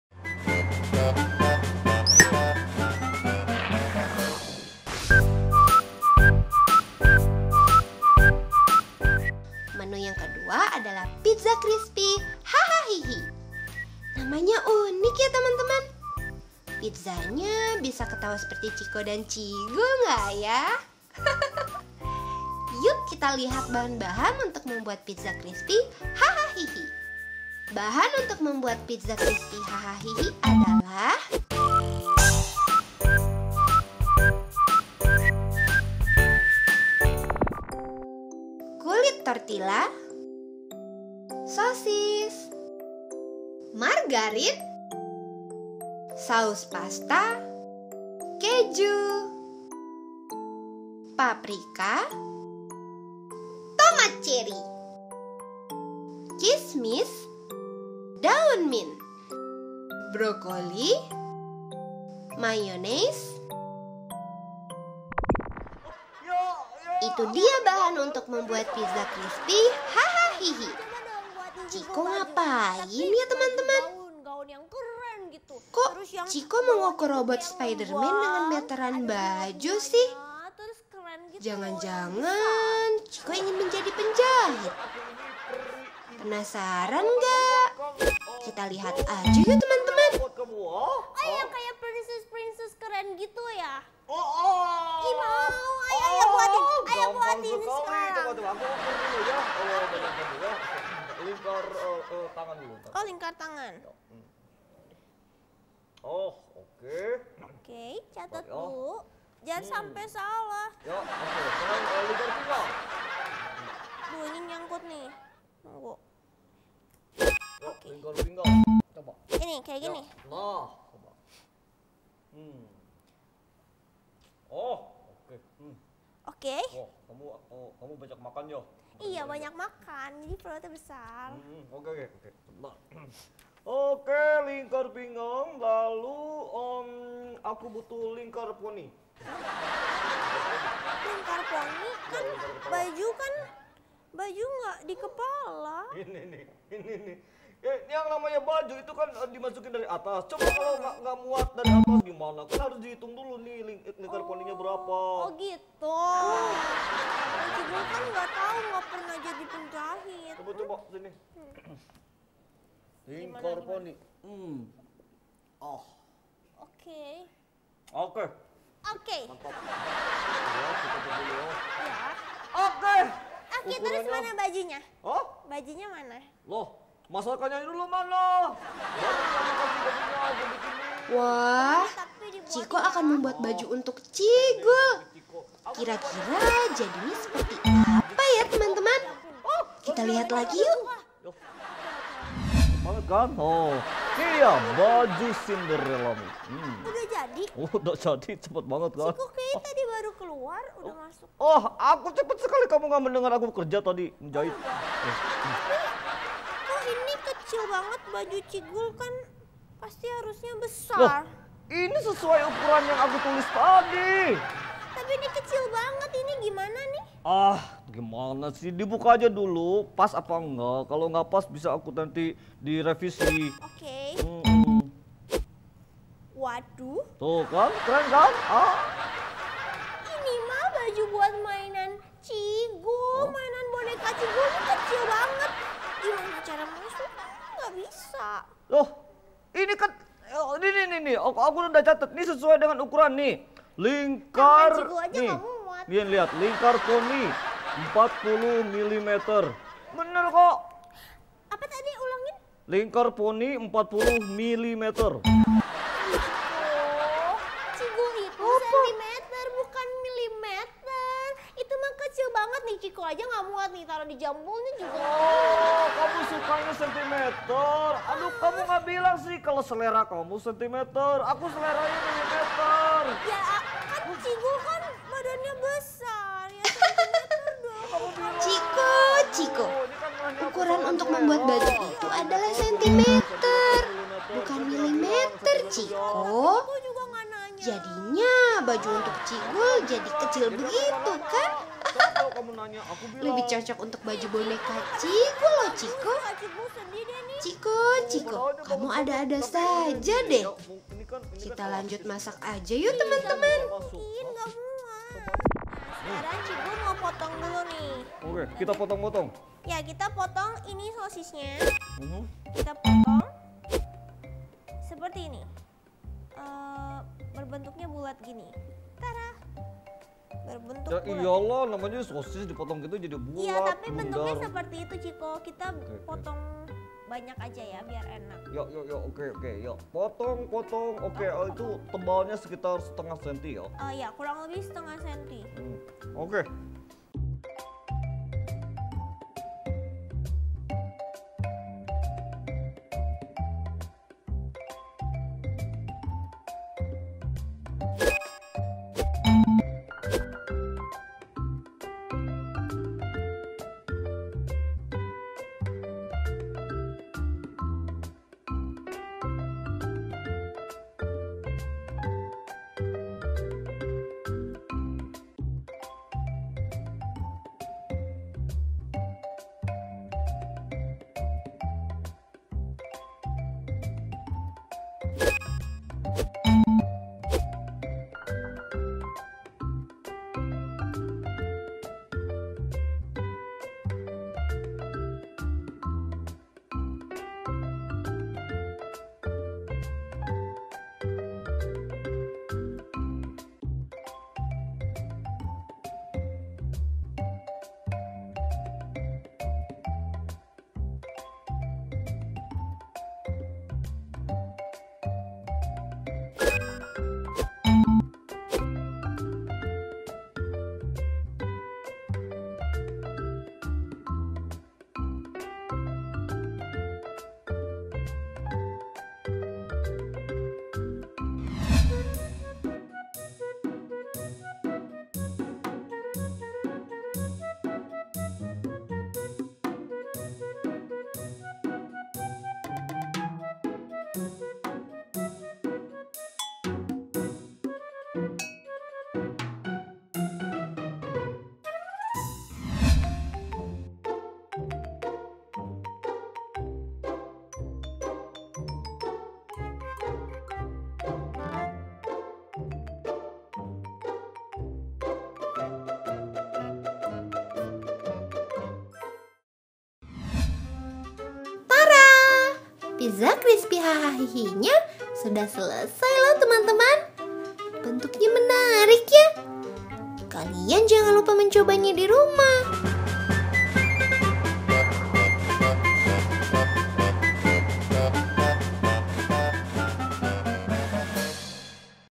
Pizza Crispy Haha Hihi Ala Mini Me
Cara membuat Pizza Crispy dengan kulit tortilla, keju, tomat cherry, brokoli, sosis, pasta tomat, mayonaise, margarin, dan paprika.